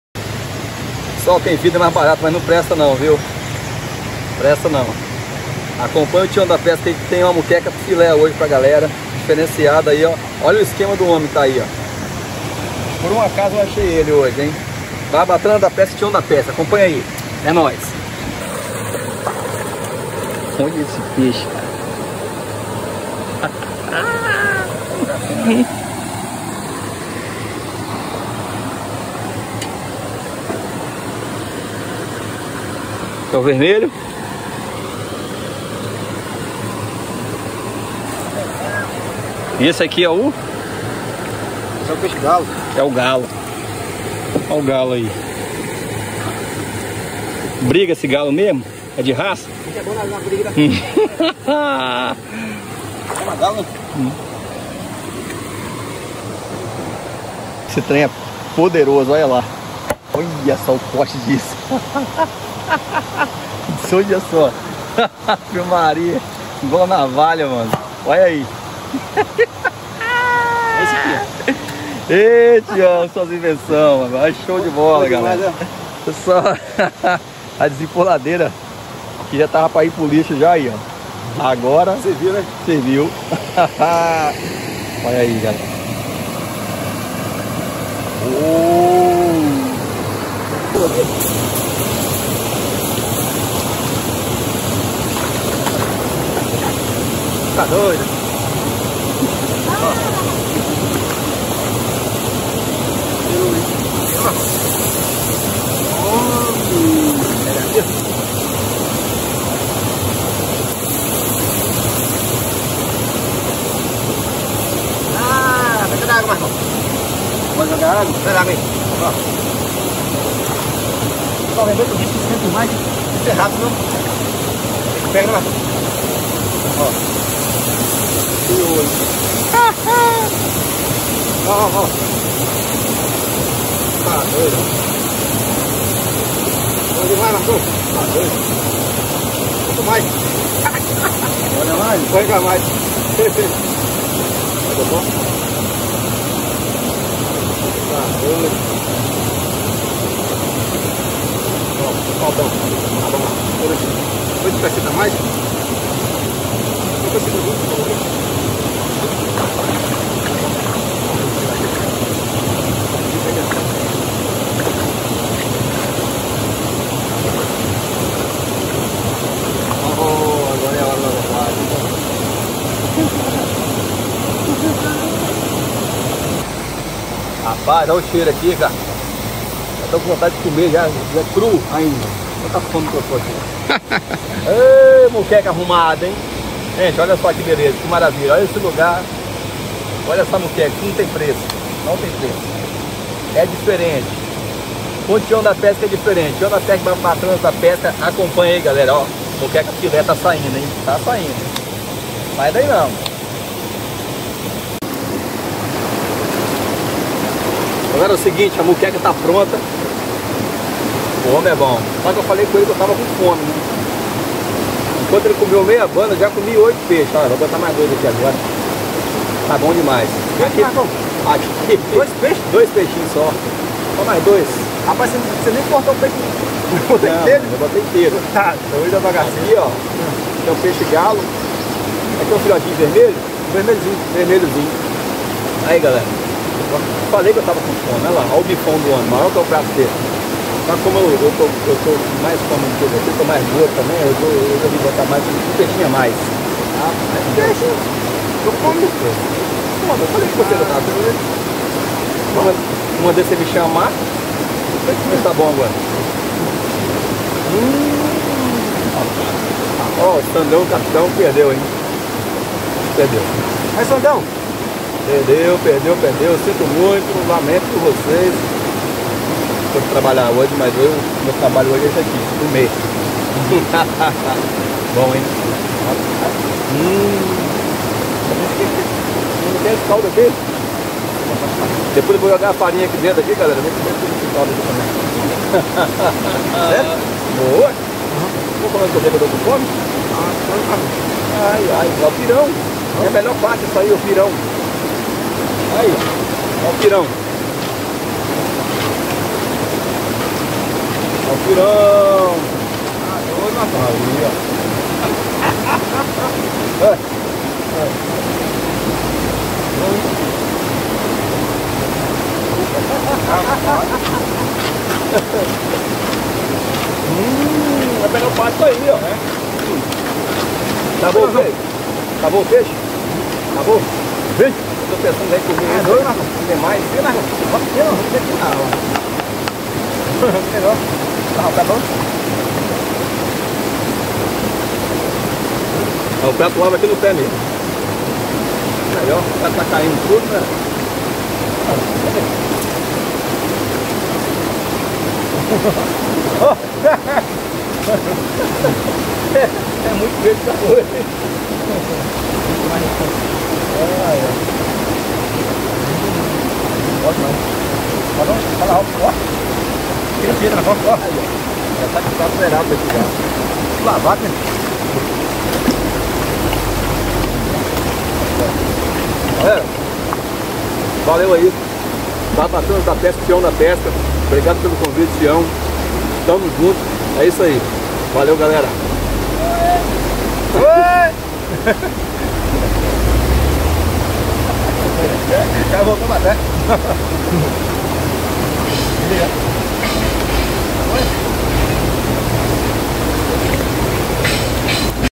PREPARAMOS UMA MOQUECA SELVAGEM.
FIZEMOS UMA MOQUECA NO RIACHO DENTRO DA MATA!!!